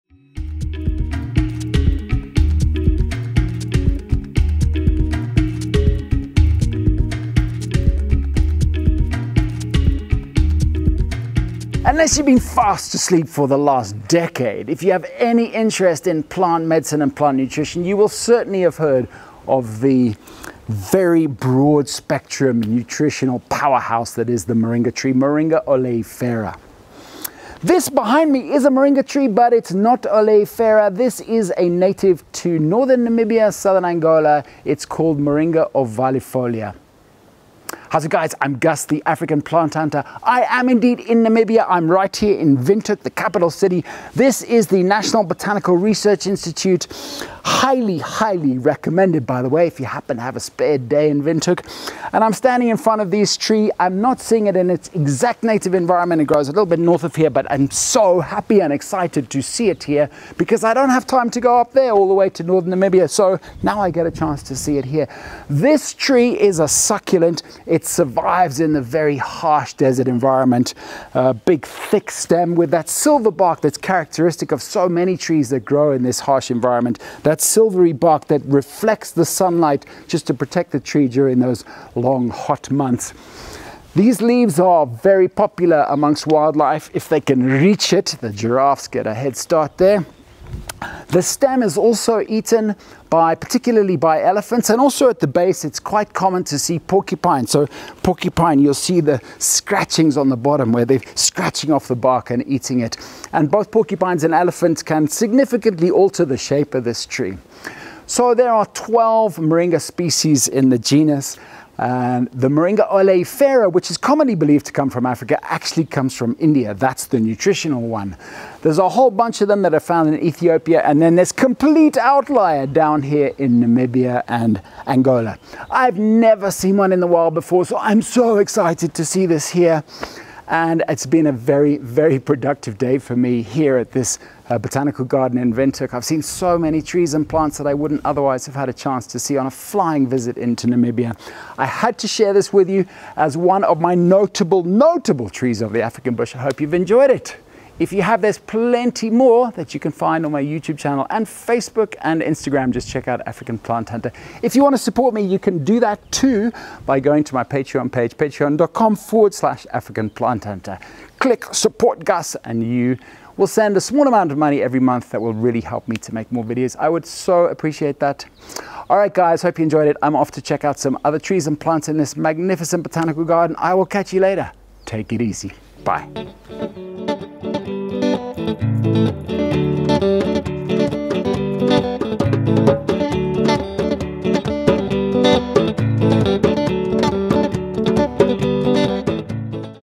Notable Trees of the African Bush E19: Ghost tree/Moringa ovalifolia
Few people haven't heard of the Moringa tree, whose leaves and seed oil are touted as something of a miracle cure for a wide range of ailments.  Although often perceived as an African species, the Moringa tree (specifically Moringa oleifera) is actually from India. But there is another, genuinely African Moringa species, which is found exclusively in Northern Namibia and southern Angola. It's called Moringa ovalifolia and, on my recent trip to Namibia, I was thrilled to see a sample in the fabulous National Botanic Garden in Windhoek.